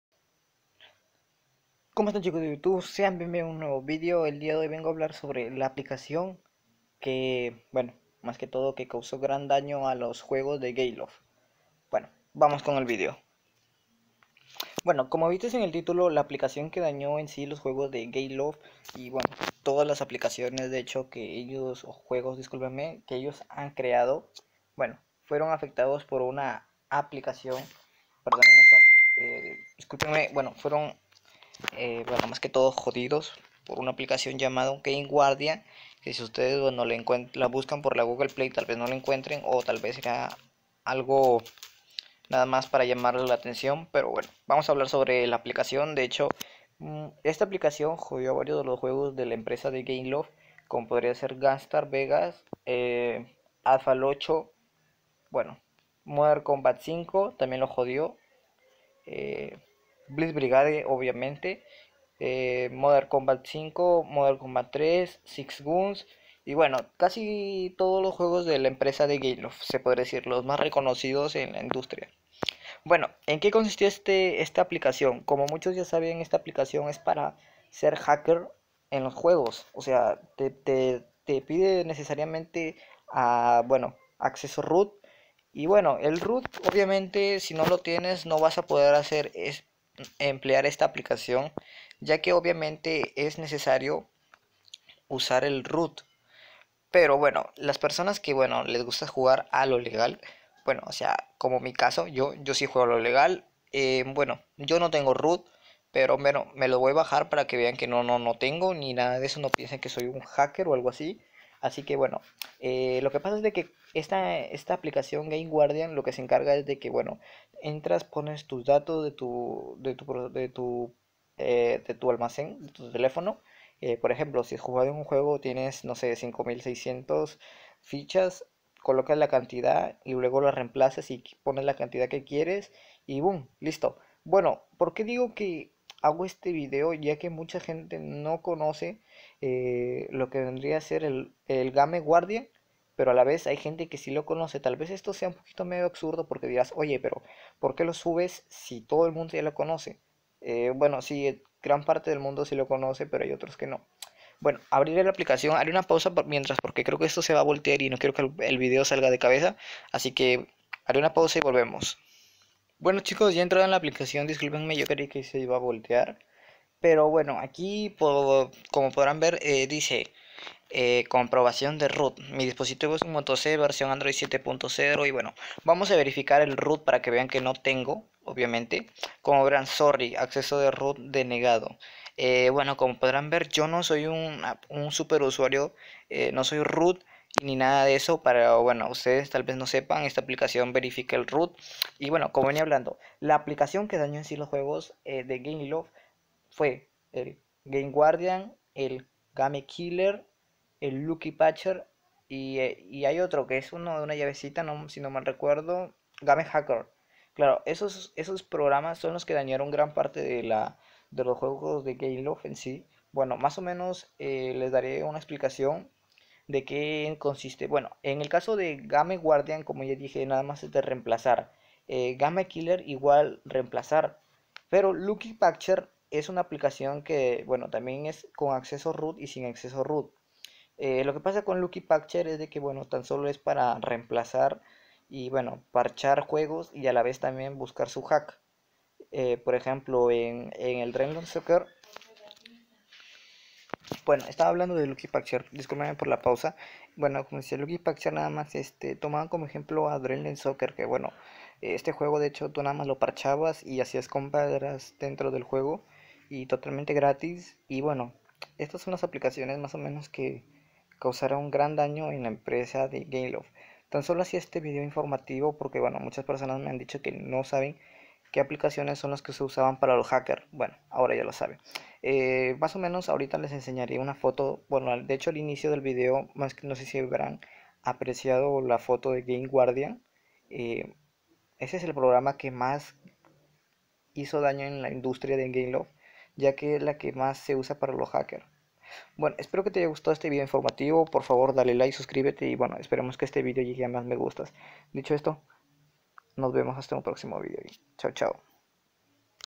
La aplicación que causó gran daño a Blitz Brigade y otros juegos de Gameloft
Este video es algo informativo sé que mucha gente conoce estos programas pero así mismo ala vez hay personas que no conocen este video esta dirigido para todos.
Metas?😎🎮♥️♥️
5likes